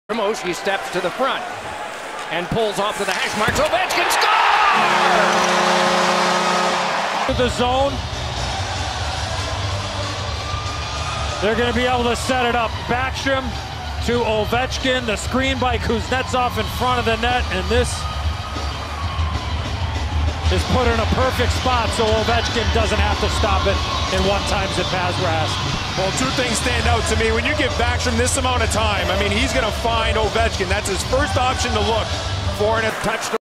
Александр Овечкин 608 гол /4.10.2018/ (начало сезона 2018-2019)
4.10.18  Александр Овечкин в матче против Бостон Брюинp забил 608 шайбу в регулярных чемпионатах НХЛ. Шайба стала 1 в сезоне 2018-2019. Овечкин догнал по количеству забитых шайб Дино Сиссарелли и разделил с ним 17 место в списке игроков забивших 500 шайб и более.